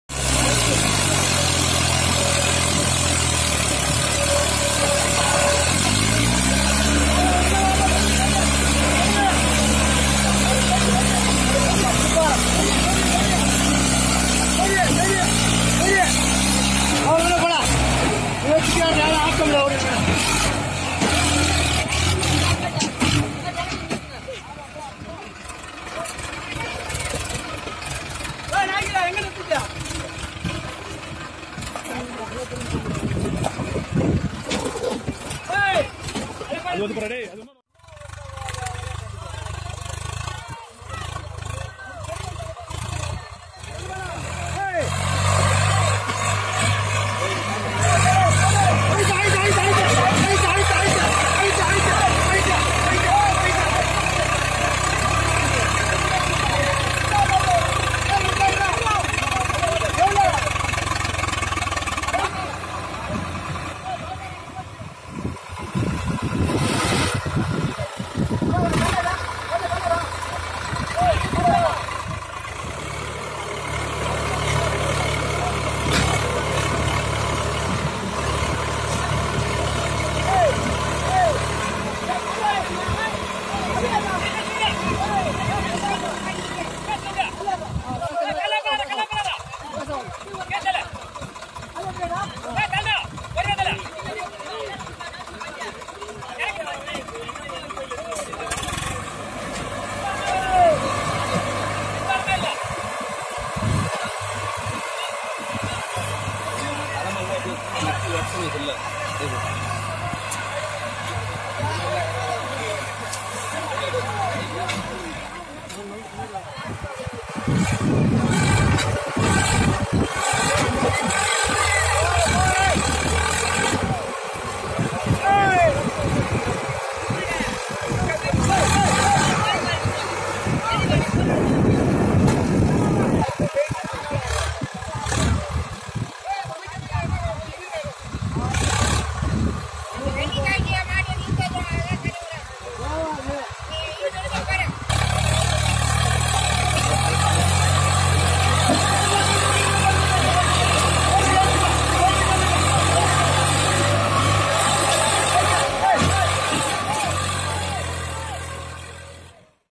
mahindra arjun vs Swaraj 744FE heavy loaded trolley pulling / come to the village /
Come to the Village
Mahindra tractor 555 DI and Swaraj tractor 744 FE trolley fully loaded by sugarcane.

tractor stuck  to heavy load.
Mahindra tractor Arjun ultra 555 DI pulling the Swaraj tractor with a heavy  loaded trolley with  full Tractor  power.